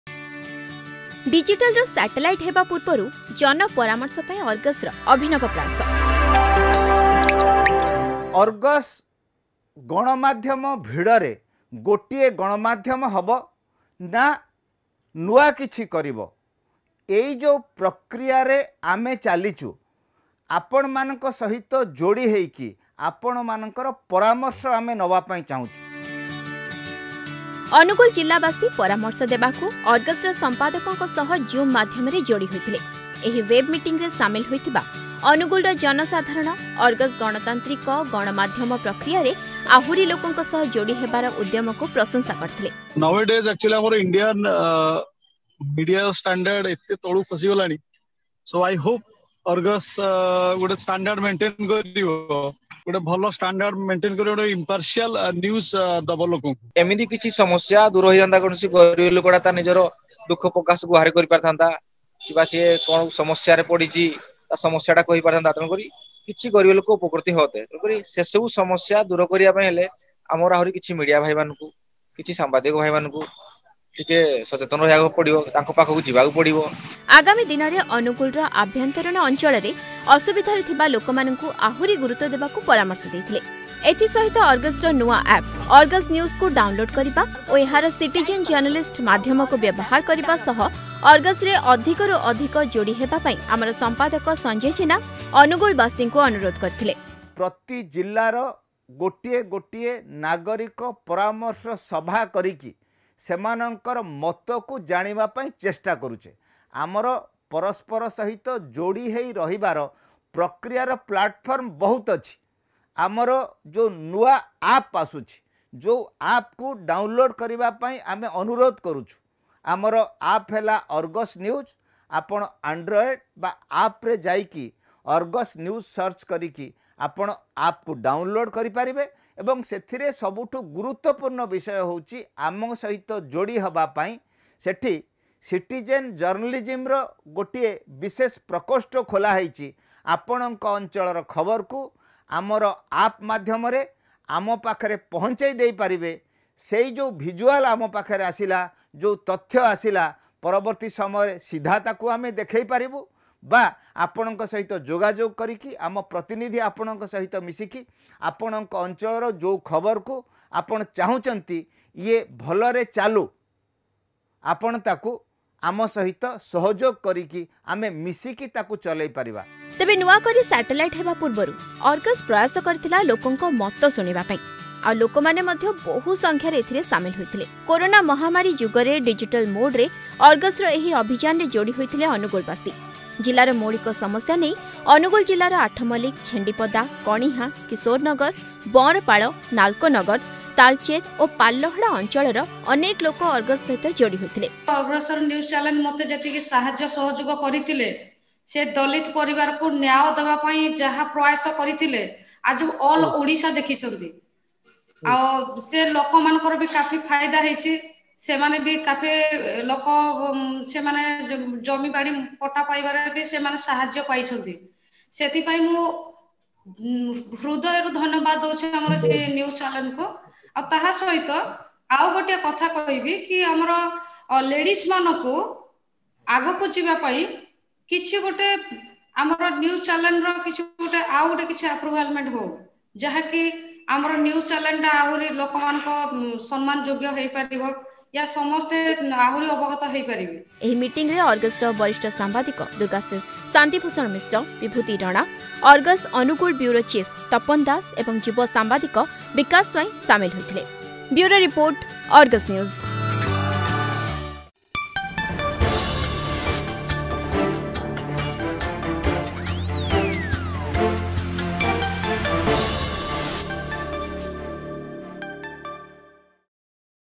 ଅର୍ଗସ ସହ ଅନୁଗୁଳ ବାସୀ
ଡିଜିଟାଲରୁ ସାଟେଲାଇଟ ହେବା ପୂର୍ବରୁ ଜନ ପରାମର୍ଶ ପାଇଁ ଅର୍ଗସର ଅଭିନବ ପ୍ରୟାସ  । ଅନୁଗୁଳ ଜିଲ୍ଲା ବାସୀ ପରାମର୍ଶ ଦେବାକୁ ଅର୍ଗସର ସମ୍ପାଦକଙ୍କ ସହ ZOOM ମାଧ୍ୟମରେ ଯୋଡି ହୋଇଥିଲେ  । ଆଗାମୀ ଦିନରେ ଅନୁଗୁଳର ଆଭ୍ୟନ୍ତରୀଣ ଅଞ୍ଚଳରେ ଅସୁବିଧାରେ ଥିବା ଲୋକମାନଙ୍କୁ ଆହୁରି ଗୁରୁତ୍ୱ ଦେବାକୁ ପରାମର୍ଶ ଦେଇଥିଲେ  ।